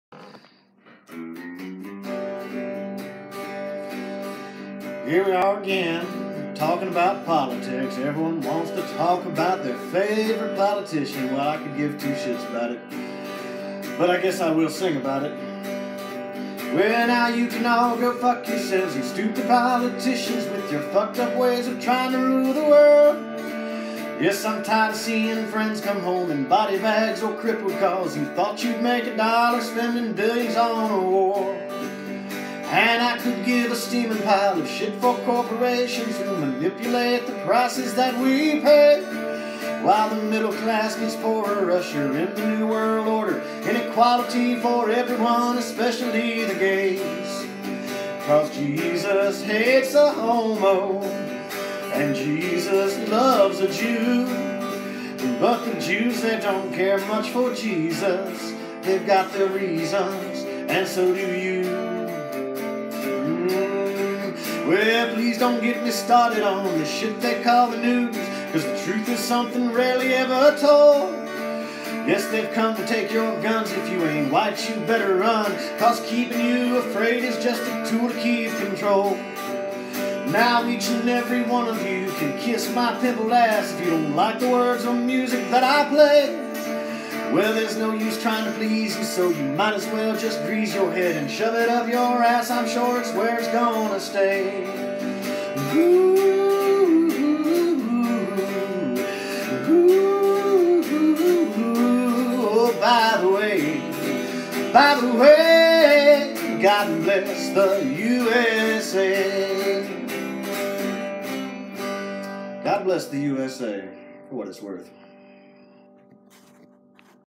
Eck - What a World
A political rant. Please do not mistake my cynicism and sarcasm for a narrow minded world view.